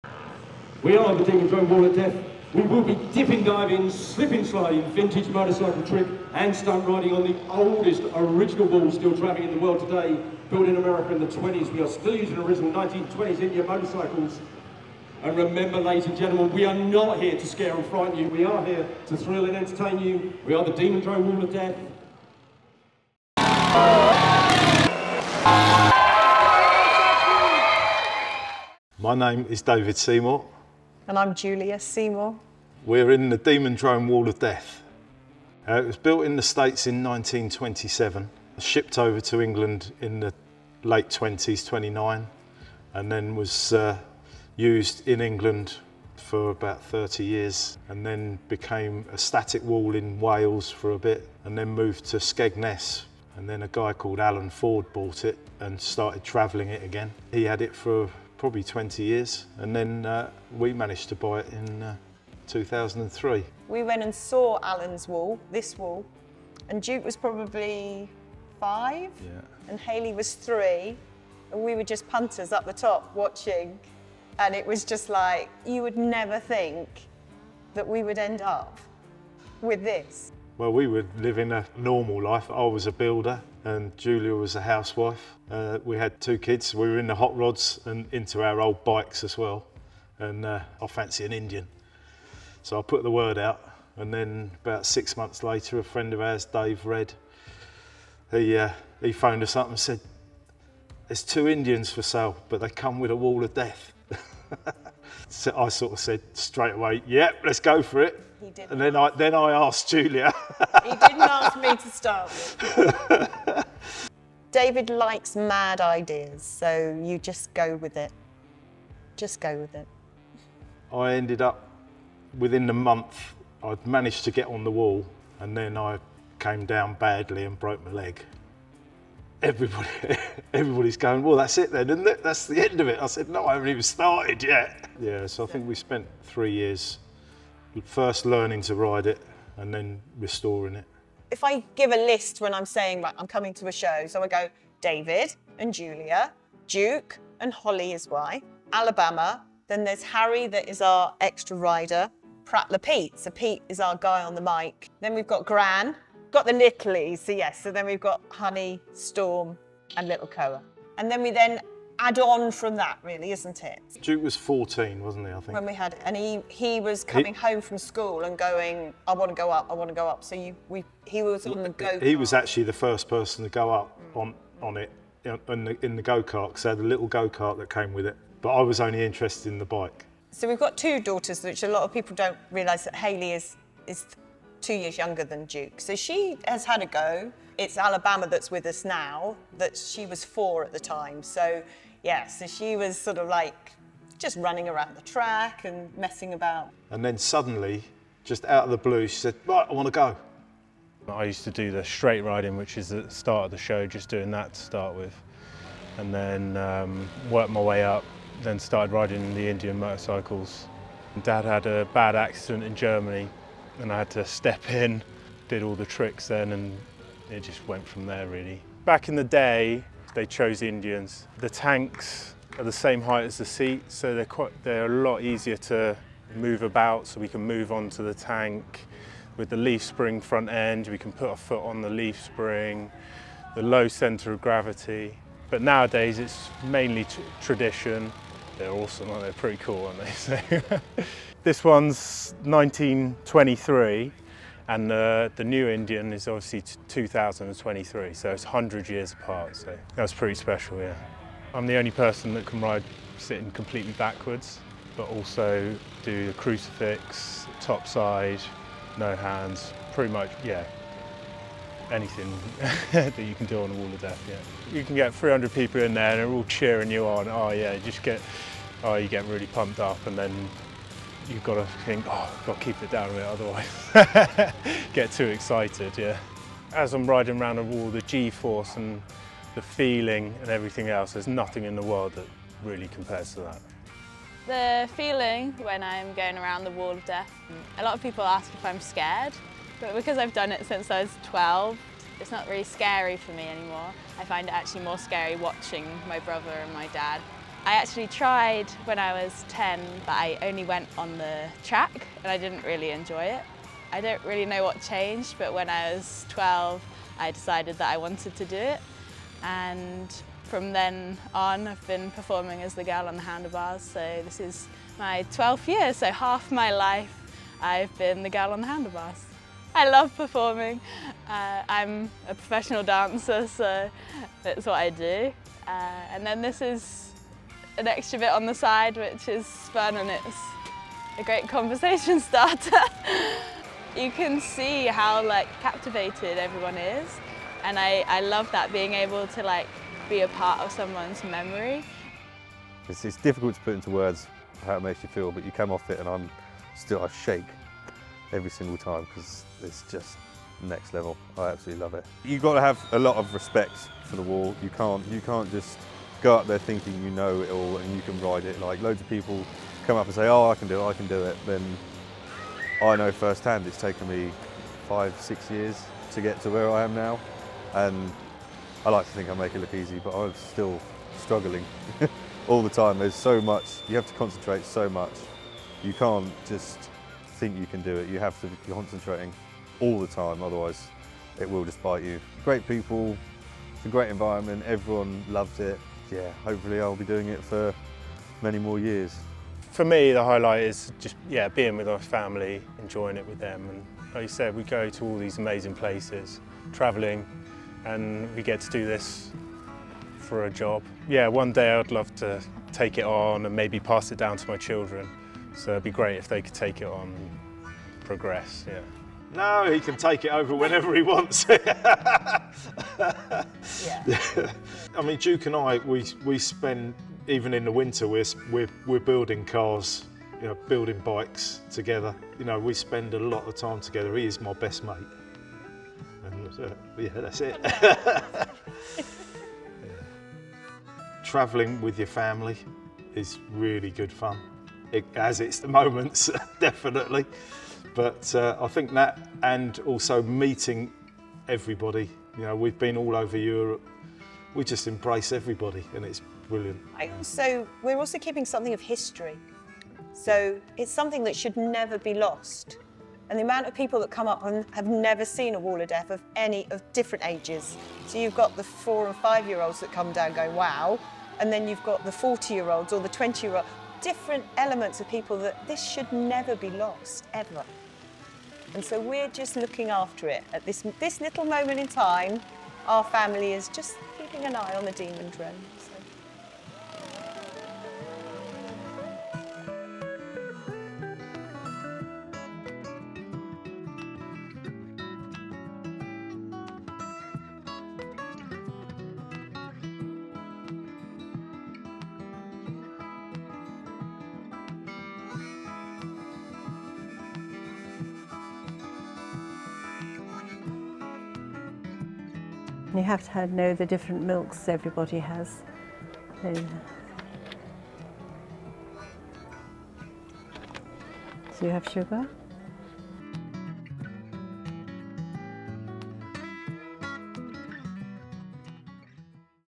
DEMON DROME - Wall of Death family documentary
Back in 2003 David Seymour was on the hunt for a vintage Indian Motorcycle. He found 2 BUT there was a catch. He could only have them if he took the Wall of Death they came with. The rest is history.

Filmed by Samson Hatae and Mark Kirkland

Edited by Samson Hatae

Follow below

00:00 - 00:20 Welcome to the wall of death
00:21 - 01:19 Introducing the Seymours
01:20 - 02:20 How did they get the wall of death?
02:21 - 03:20 Meet the family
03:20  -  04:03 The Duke
04:04 - 04:38 100 year old Indian Motorcycle
04:39 - 06:45 "No other feeling in the world"
06:46 - 07:58 "He can take it on whenever he likes"
07:59  - 08:22 Super 8 outro
08:23 - 08:37 Cup of tea with Nan